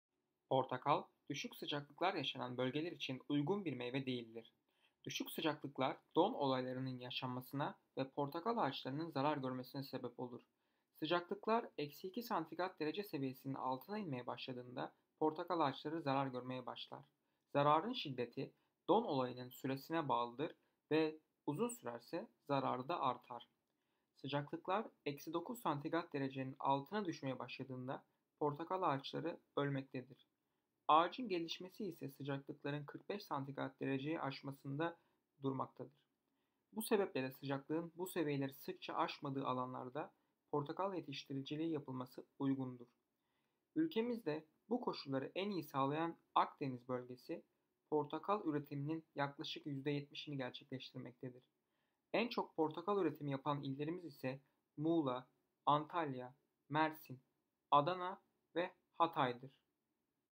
🍊Portakal Türkiye'de Nerede Yetişir? | Ne Nerede Yetişir #3
Türkiye'de portakal yetiştiriciliği hangi il ve ilçelerde yapılmaktadır?